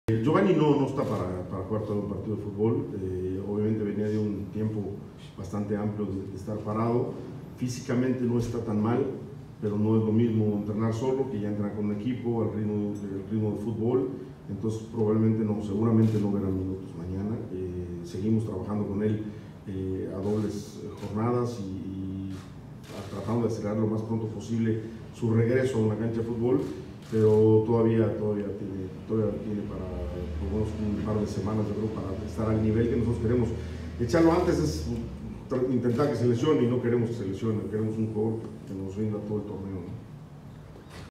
GIO DEBUTARÁ EN DOS SEMANAS
‪NO DEBUTARÁ MAÑANA‬

‪@OficialGio seguirá con su preparación, no está para jugará mañana, aquí las palabras de @MiguelHerreraDT 👇🏼‬